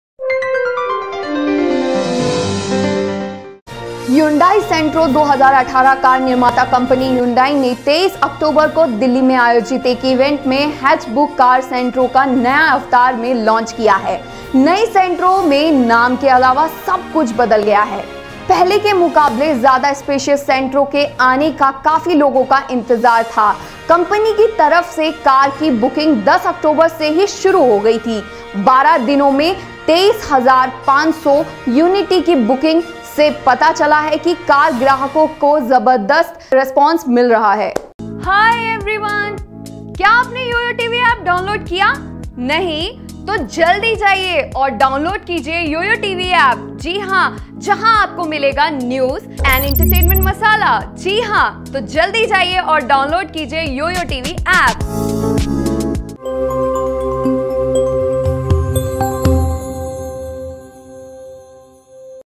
Hyundai Santro 2018 Car Maker Hyundai Launches Hatchback Car Centro In A New Avatar | YOYO TV Hindi
Hyundai Santro 2018 car maker Hyundai launches Hatchback Car Centro in a new avatar on October 23 at an event held in Delhi. Everything has changed in addition to the name in the new Centro . A lot of people were waiting for the arrival of more Spicey Centro than before. Bookings from the company started on October 10. Bookings of 23,500 units in 12 days showed that the car is getting a tremendous response to customers. Prior to launch, the new Centro remains in the headlines is special in many ways from the old car.

Good chance if you are planning a car
If you are planning to take a new centro then you have a good chance. Earlier, the company had fixed the amount of Rs 11,100 for its booking. This offer is still being offered by the company. If you book the online car with HDFC Bank debit or credit card, then it will get 10 percent cash back. That is, you have to pay a total of Rs 10,100
So what can you do by booking a new centrally and quadruple the happiness of the upcoming Diwali? Online booking can be done from the company's website till November 30. Let me tell you one more thing at the hands of the HDFC Bank cashback offer only when you take an auto loan from HDFC. The new Santro has a 1.1-liter petrol engine, it also has the option of CNG. The new Centro has also been given the option of automatic.
re powerful than ever before in New Centro
The length and width of the new Centro are also more than before, which makes it look more strong than ever. The car has a touchscreen input system of 17.64 centimeters. Apart from this, Apple Car Play, Android Auto Factors will also be available. The new Centro also has a rear AC vent. All Variants of the car have been given ABS and Driver Airbags. The petrol engine has a power of 69 PS at 5500 rpm. The power of the CNG engine is at 59 ps. The company claims 5-speed manual transmission and AMT version of 20.3 km per liter mileage. The car's CNG variant will mileage 30.48 km/kg. The car is launched in 5 variants, Delight, ERA, Magna, Sports, and ASTA variants.